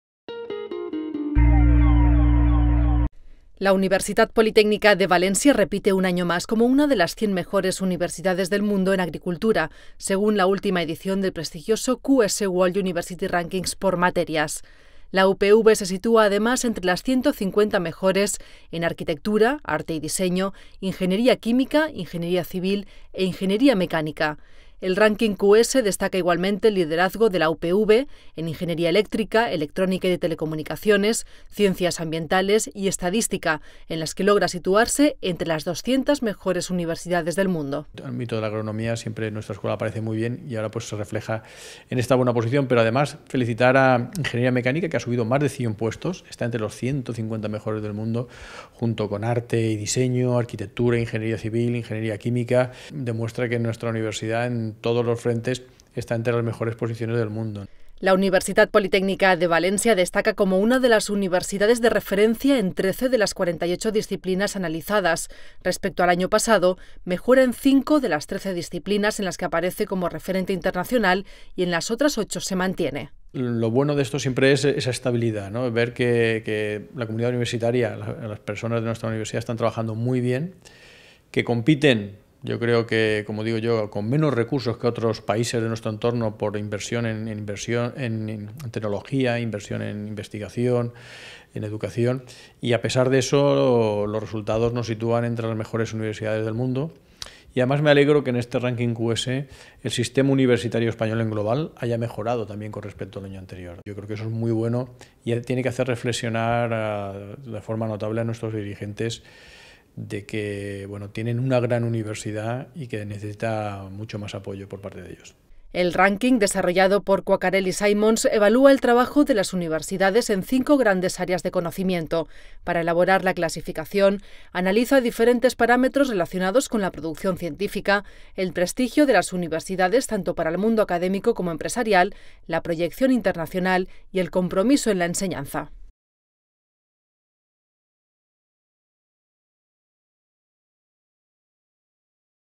Ranking QS 2018: la UPV entre las 100 mejores en Agricultura - Noticia @UPVTV, 28-02-2018
La Universitat Politècnica de València repite un año más como una de las cien mejores universidades del mundo en Agricultura, según la última edición del prestigioso QS World University Rankings por materias.

La UPV se sitúa además entre las 150 mejores en Arquitectura; Arte y Diseño; Ingeniería Química; Ingeniería Civil e Ingeniería Mecánica.

El ranking QS destaca igualmente el liderazgo de la UPV en Ingeniería Eléctrica, Electrónica y de Telecomunicaciones; Ciencias Ambientales; y Estadística, en las que logra situarse entre las 200 mejores universidades del mundo.

La Universitat Politècnica de València destaca como una de las universidades de referencia en 13 de las 48 disciplinas analizadas.

Respecto al año pasado, mejora en cinco de las trece disciplinas en las que aparece como referente internacional y en las otras ocho se mantiene.

El ranking, desarrollado por Quacquarelli Symonds, evalúa el trabajo de las universidades en cinco grandes áreas del conocimiento. Para elaborar la clasificación, analiza diferentes parámetros relacionados con la producción científica, el prestigio de las universidades tanto para el mundo académico como empresarial, la proyección internacional y el compromiso en la enseñanza.